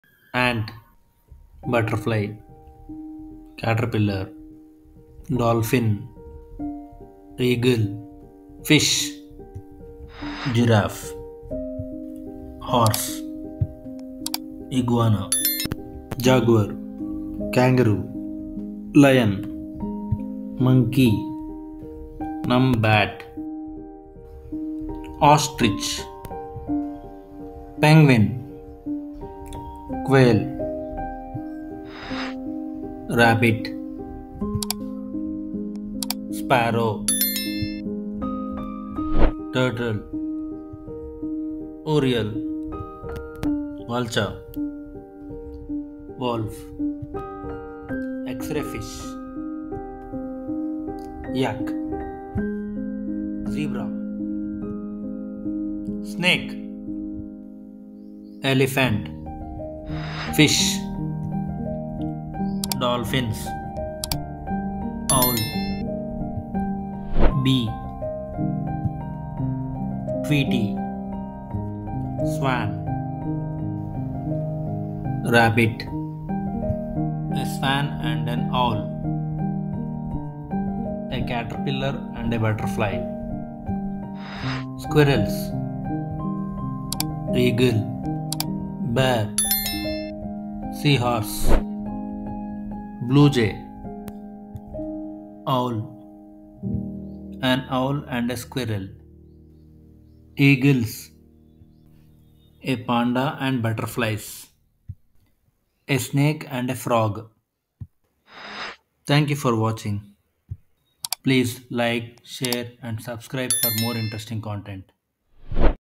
Learn Animals in Just Two Minutes 🌍🐾 | Learning for Kids | Animals Names pronunciation
🦁🐼🦓 Dive into the fascinating world of wildlife with our fast-paced journey through the Animal Kingdom! From the majestic roar of lions to the playful antics of pandas, join us on an adventure to learn about a diverse array of creatures in just two minutes. This whirlwind tour promises to leave you amazed and informed. Don't miss out – hit play and embark on a brief yet captivating exploration of the animal wonders that share our planet! 🌿✨